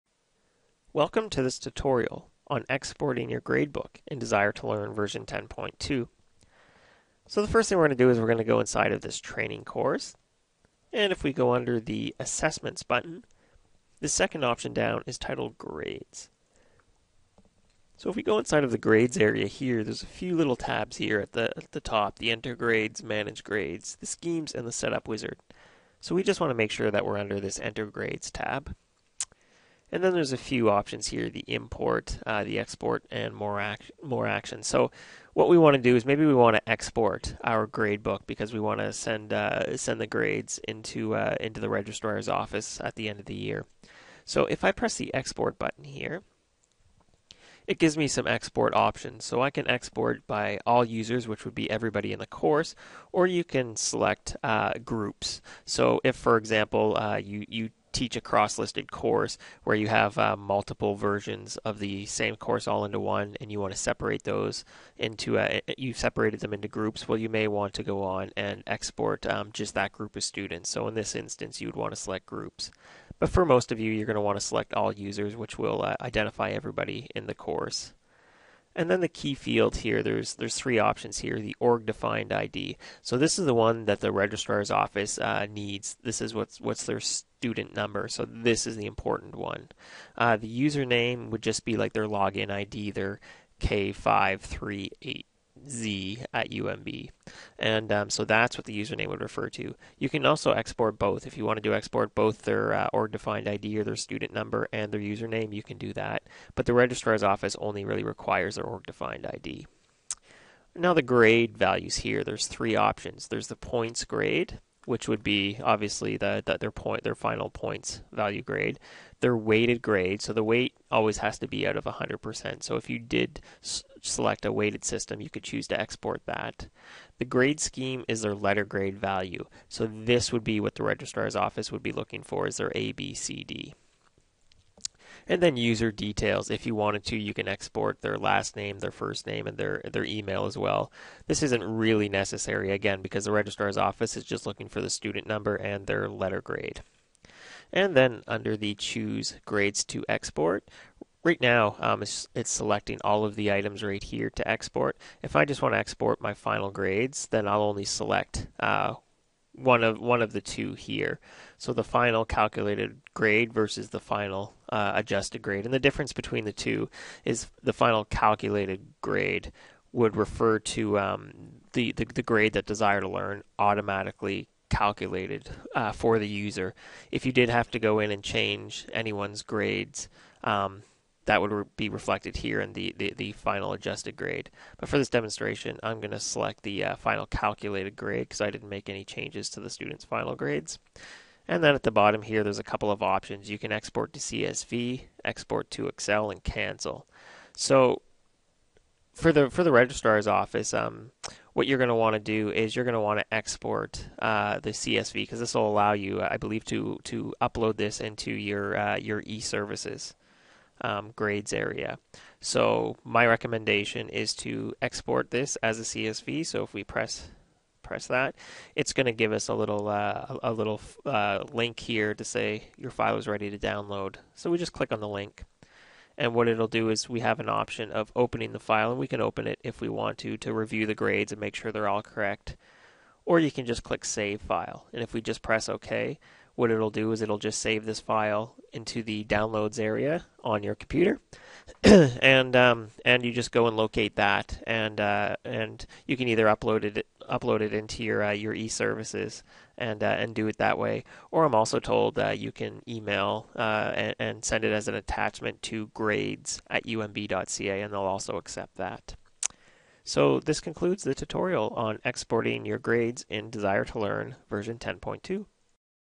Exporting Grades in D2L
A step by step guide on how to export your gradebook in D2L version 10.2